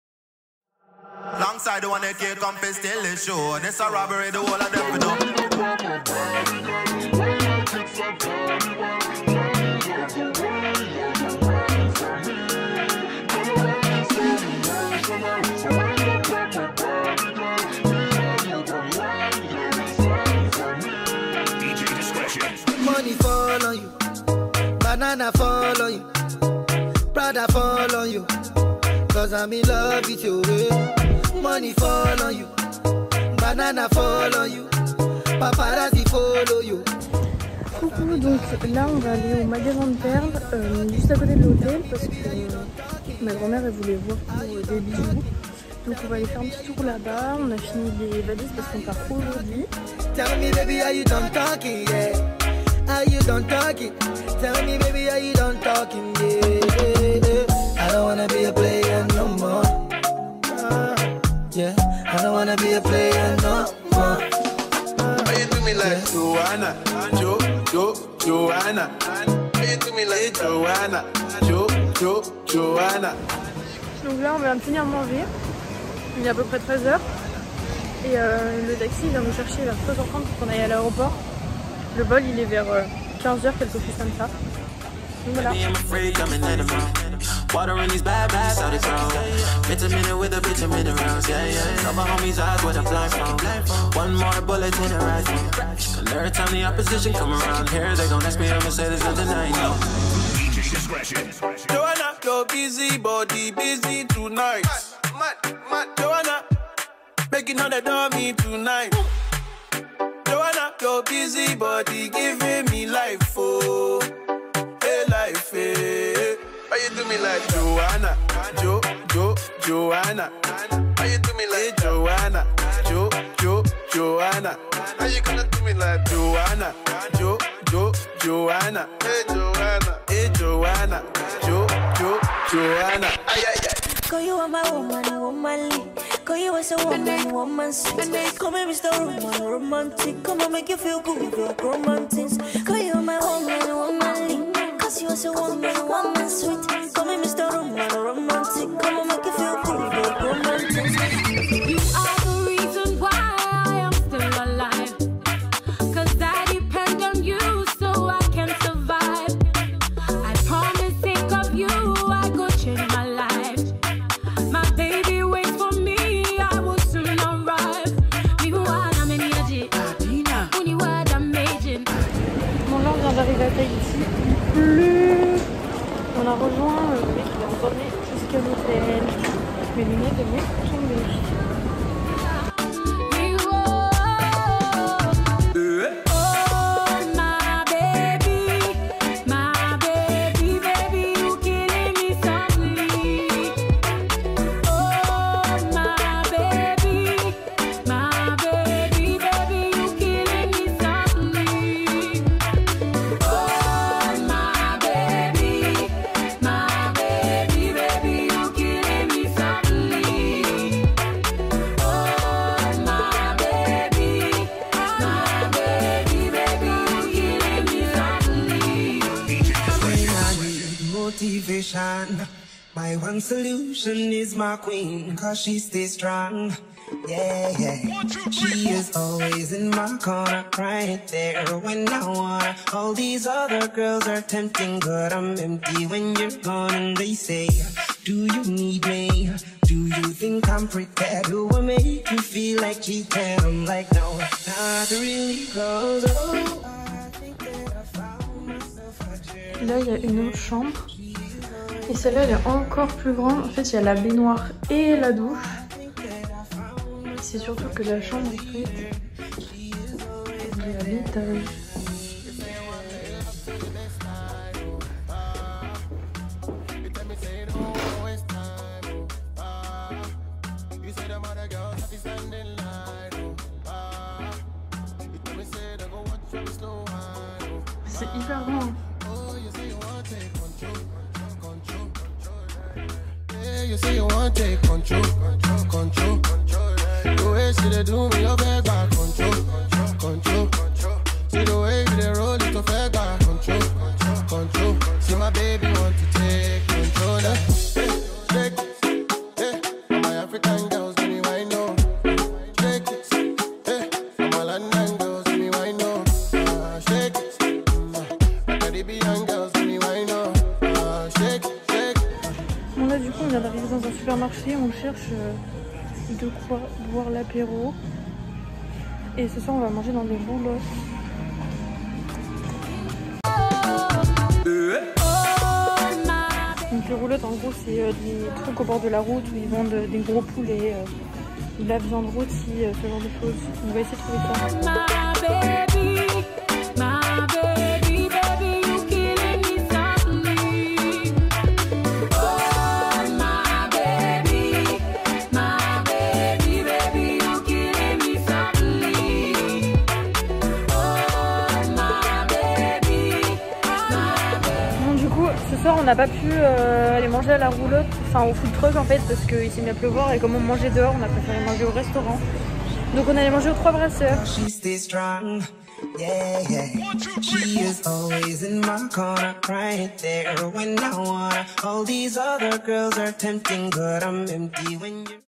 VLOG 19 - On débarque à Tahiti 🌴
Je vous partage mes vidéos et vous emmène avec moi en voyage✈️

Dans le vlog d’aujourd’hui :

Aujourd’hui on part de Rangiroa en direction de Papeete, la capitale, à Tahiti 🌴

N’hésitez pas à commenter, partager et liker la vidéo !

Les questions qui reveinnent le plus :

• je filme avec un IPhone 11
• mes réseaux :